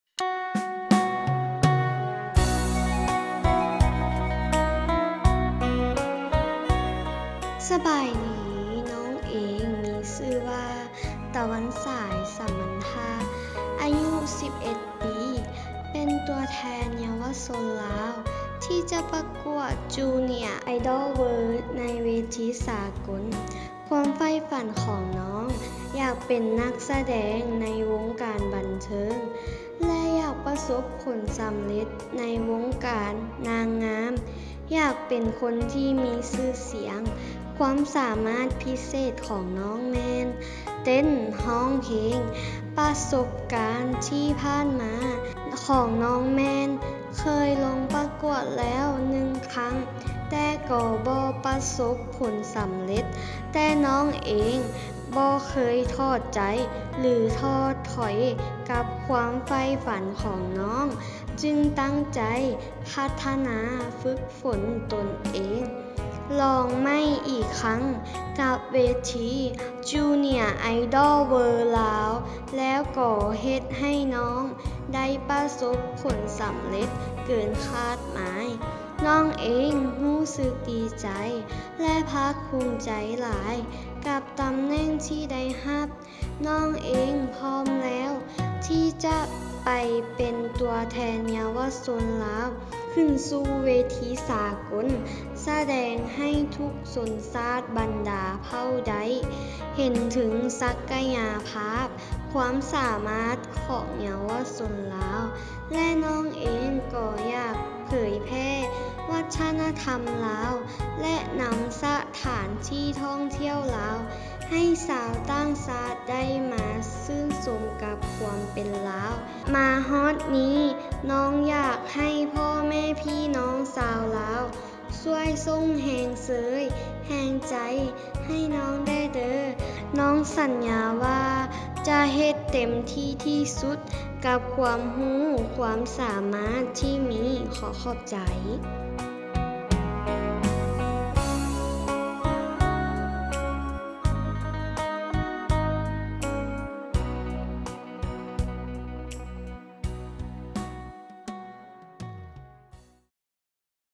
ຕະວັນສາຍ 1st Runer up Miss junior idol world laos 2022 (VTR interview)
ຕອນນີ້ ນ້ອງກຳລັງປະກວດຢູ່ປະເທດໄທ ຕັ້ງແຕ່ 8-13 ມິຖຸນາ 2022 ຊ່ວຍເປັນກຳລັງໃຈໃຫ້ນ້ອງດ້ວຍ.

TK entertainment / News star channel
ຕິດຕໍ່ງານສະແດງ ຫລື ຖ່າຍທຳມິວຊິກວິດີໂອ ໂທ ຫລື ວອດເອັບ: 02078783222